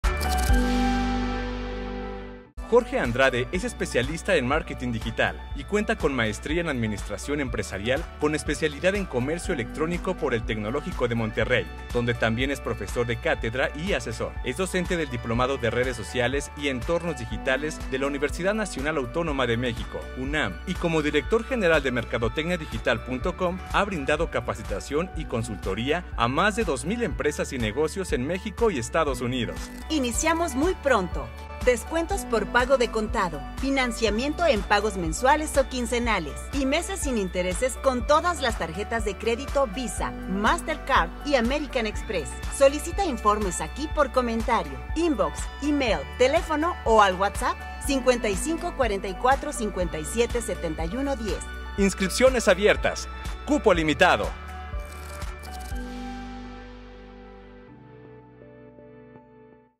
M.A.E. Jorge Andrade
Jorge Andrade es especialista en marketing digital y cuenta con maestría en Administración Empresarial con especialidad en Comercio Electrónico por el Tecnológico de Monterrey, donde también es profesor de cátedra y asesor. Y como Director General de MercadotecniaDigital.com ha brindado capacitación y consultoría a más de 2,000 empresas y negocios en México y Estados Unidos